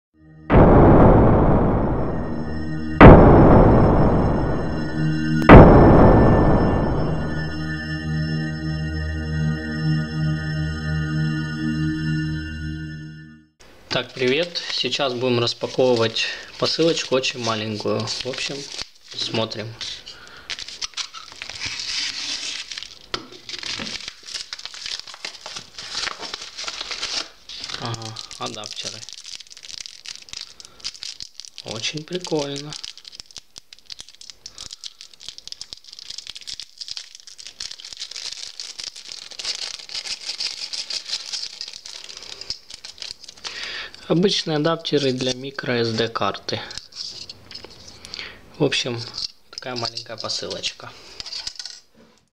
3 кард ридера (адаптера micro sd) Посылка из Китая
Это очень маленькая и дешевая посылочка. Так как у меня то и дело,что ломаются ридеры карт памяти -заказал из в Китае. Стоимость была около 4 грн за 3 штуки. с доставкой в Киев. Все проверил - оказались рабочие.

Все вопросы по товарам пишите в личку админу в разделе контакты